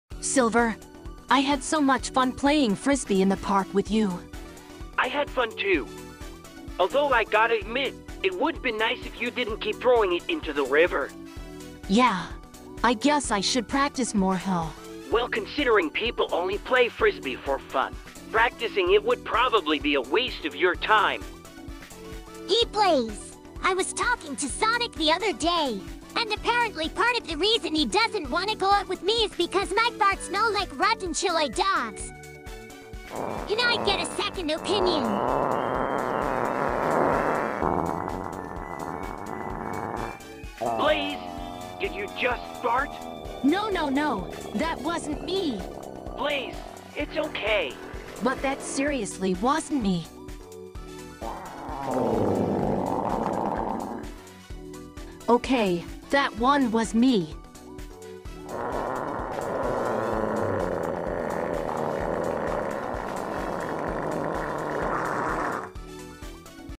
Sonic Girls Farting Comics: Phoning It In (voiced)
Finally a new one done, did this between work and other RL things. Hope the next one doesn't take as long. Happy 2023 everyone. ^^